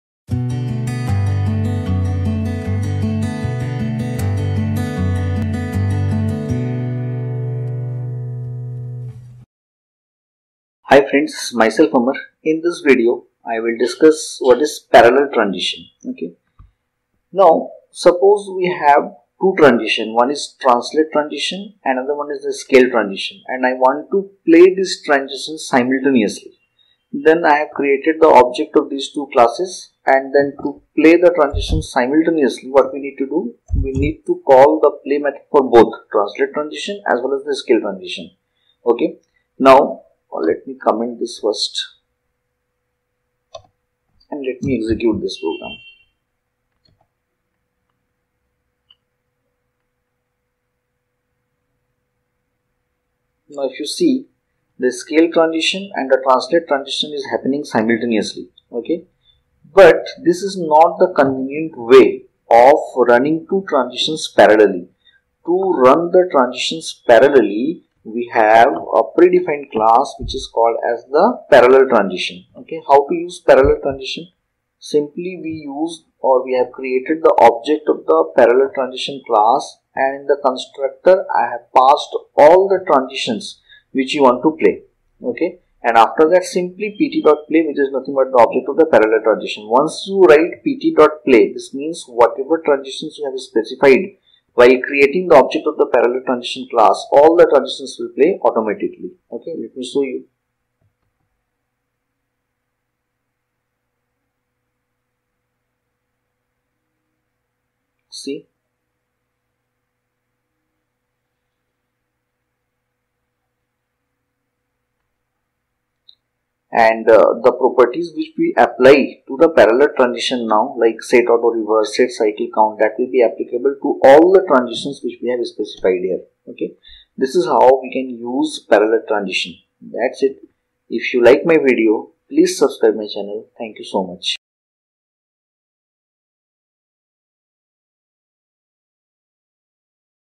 JAVAFx Tutorial 33-Parallel Transition
In this video JavaFx Parallel transition has been discussed.